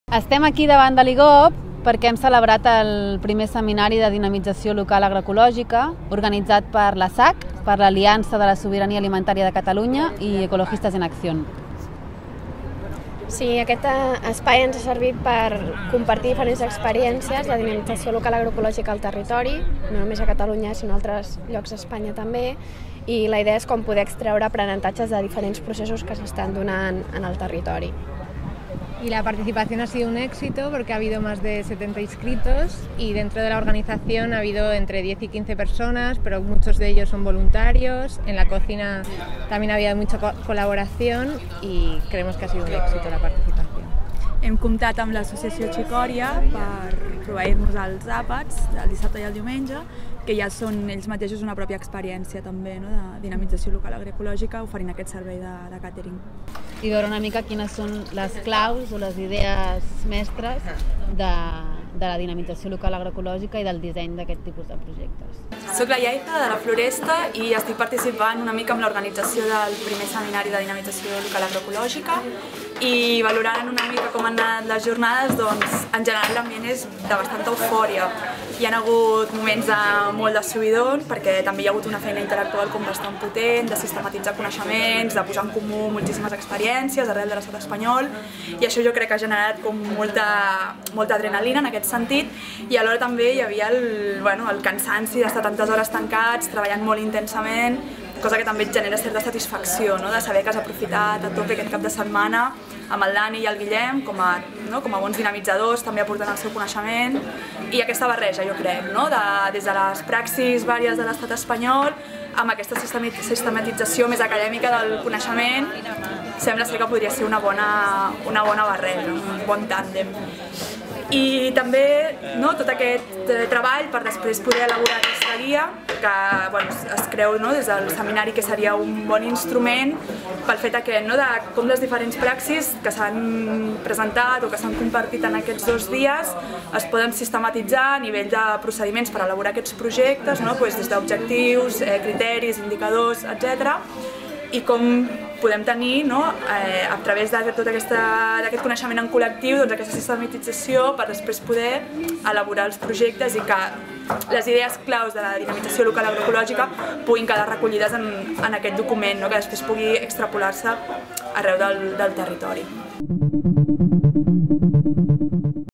Contrainfos 30 I Seminari de dinamització local Agroecológica
I Seminario de Dinamización Local Agroecológica, realitzat per Ecologistes en Acció i Asac: associació per la Sobirania Alimentària a Catalunya